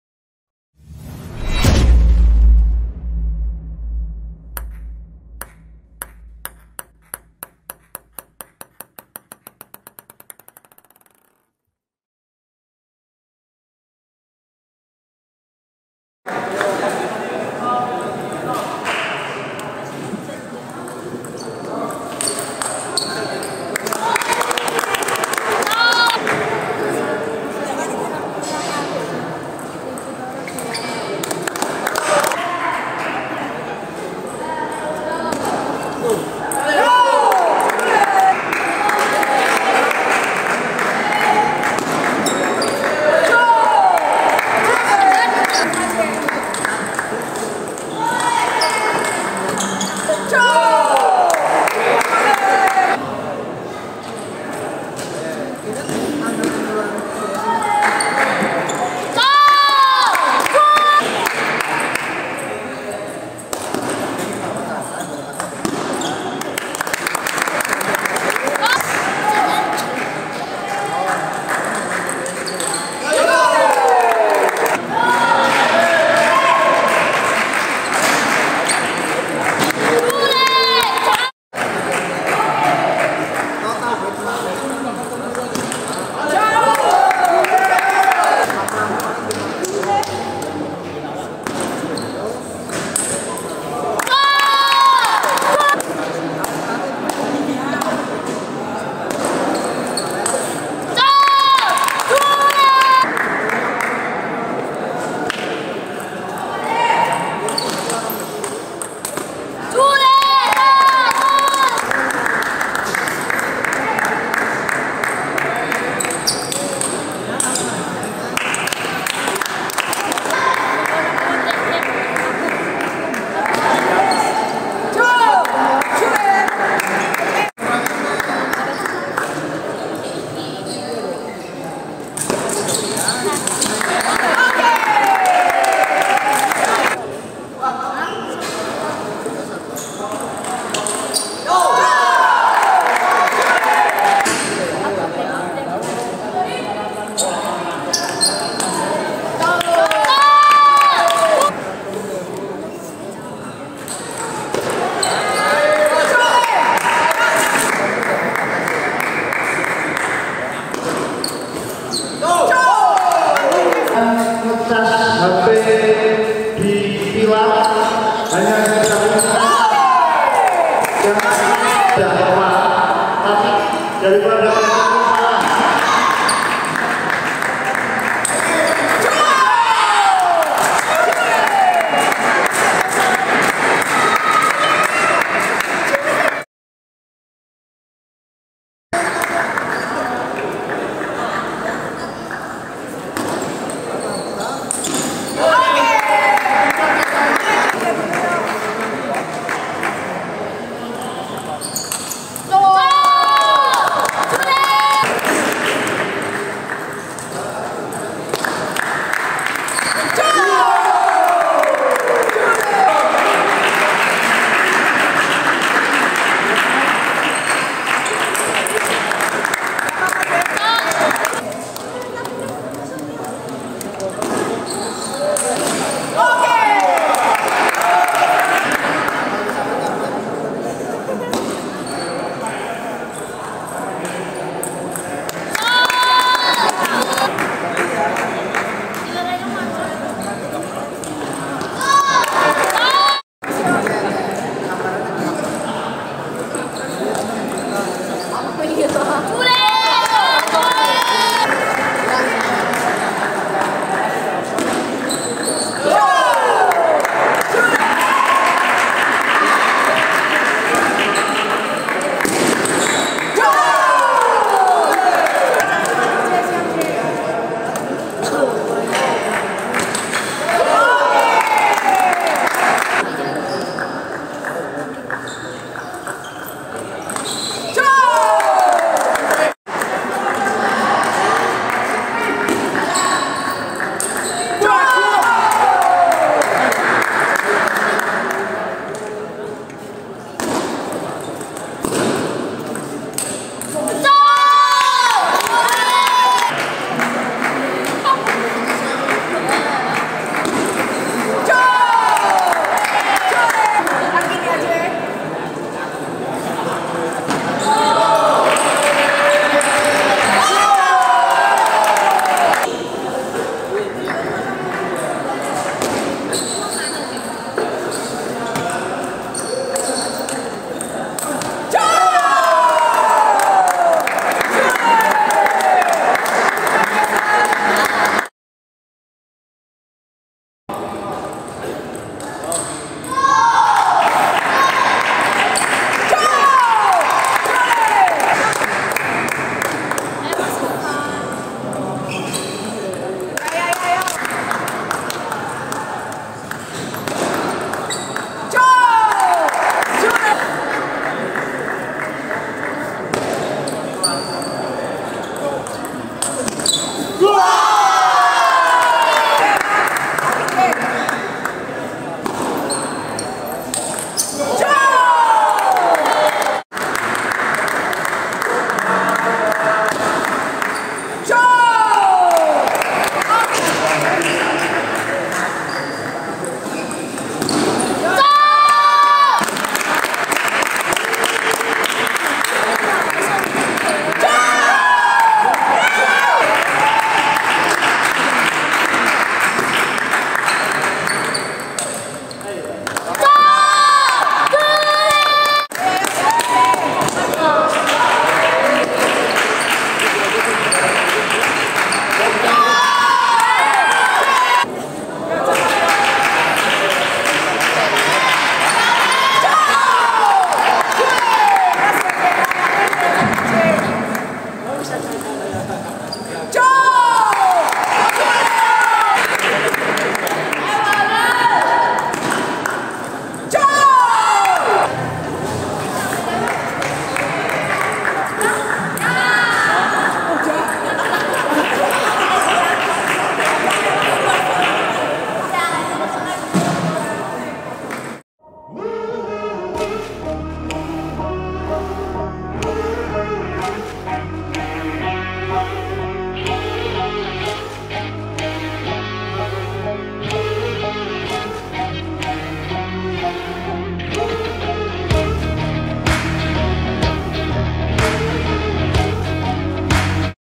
JASON (SPSO) vs ARFAN (Annell) 🏓🏓 Semifinal Perorangan U18 Putra Piala Binora XXV
Kejuaraan Terbuka Tenis Meja Piala Binora XXV 2024, diselenggarakan di GOR SMAN 3 Klaten, Jawa Tengah, Indonesia.

Jason
Nittaku Irodori Gyo-En
Black : Nittaku Hurricane
Red : Nittaku Fastarc G1